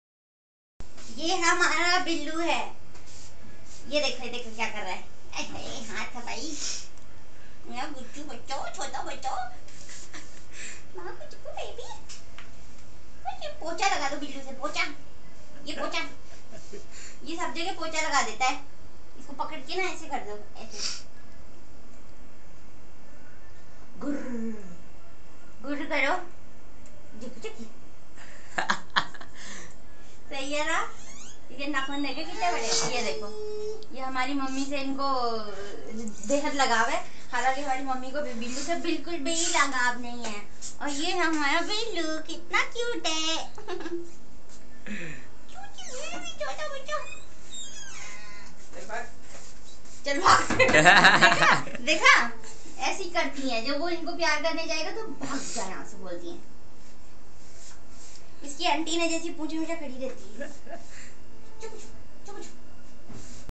Mah Billus 1
This is my Cute Billu, but now he is lost :( I really love him I hope he comes back one day